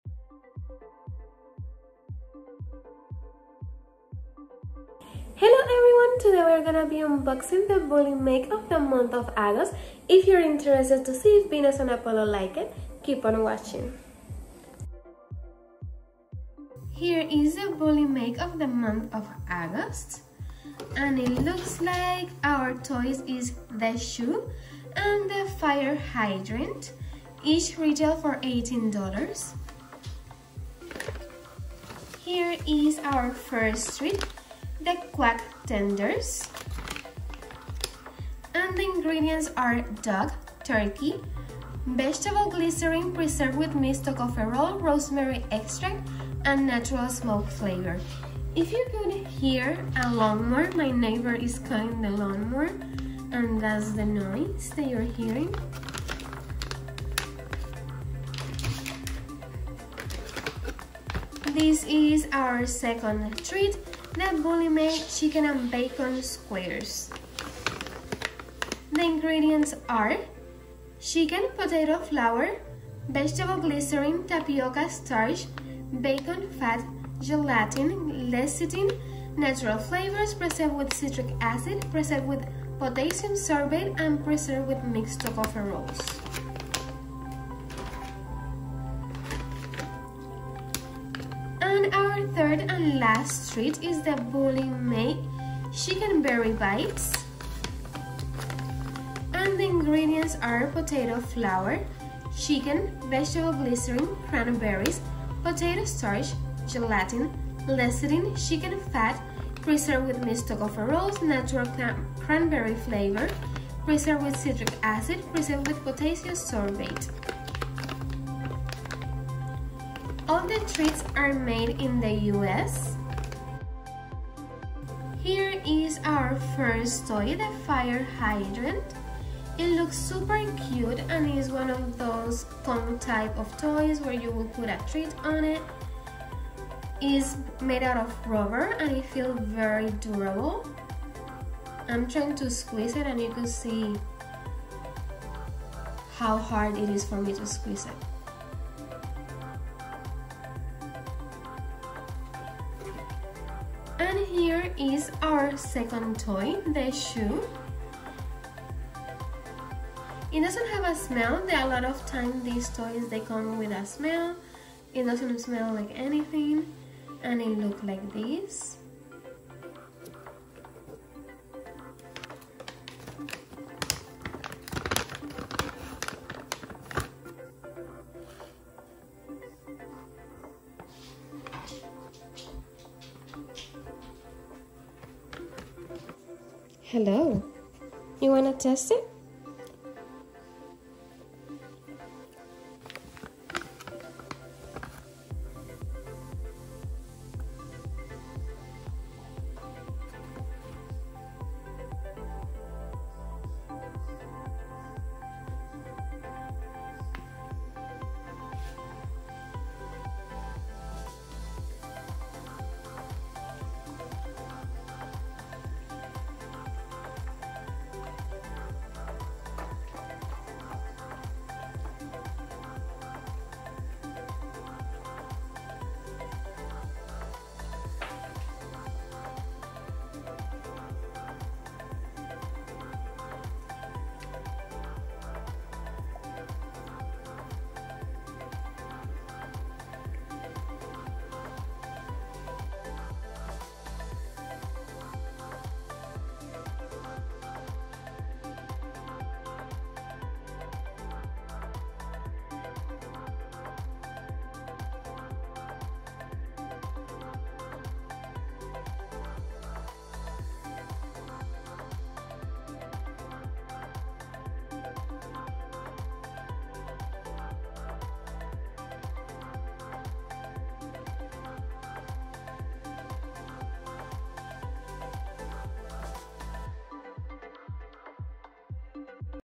August BullyMake Box 2020 unboxing
The bullymake box was sent to us for a review but this video is not sponsored.